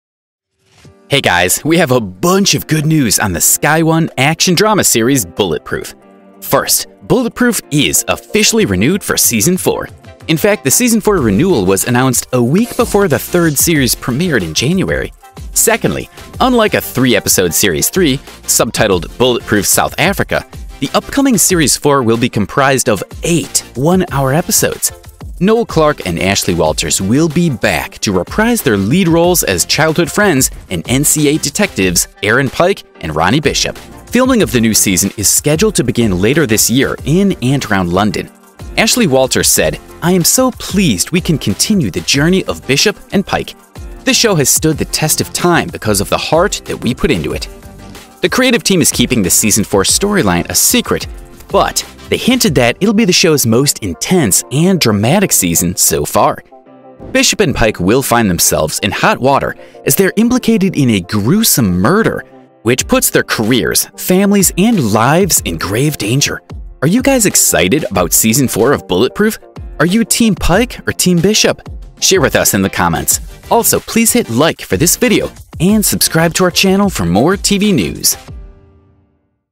BULLETPROOF Season 4 Release Set for 2022: Bishop and Pike’s Most Dramatic Season Yet
Hey, guys! We have a bunch of good news on the Sky One action drama series Bulletproof. First, Bulletproof is officially renewed for Season 4! In fact, the Season 4 renewal was announced a week before the third series premiered in January. Secondly, unlike a 3-episode Series 3, subtitled Bulletproof: South Africa, the upcoming Series 4 will be comprised of EIGHT one-hour episodes. Noel Clarke and Ashley Walters will be back to reprise their lead roles as childhood friends and NCA detectives Aaron Pike and Ronnie Bishop. Filming of the new season is scheduled to begin later this year in and around London. Ashley Walters said: “I am so pleased we can continue the journey of Bishop and Pike. This show has stood the test of time because of the heart we put into it.” The creative team is keeping the Season 4 storyline a secret, but they hinted that it’ll be the show’s most intense and dramatic season so far. Bishop and Pike will find themselves in hot water as they are implicated in a gruesome murder, which puts their careers, families and lives in grave danger. Are you, guys, excited about Season 4 of Bulletproof? Are you team Pike or team Bishop? Share with us in the comments!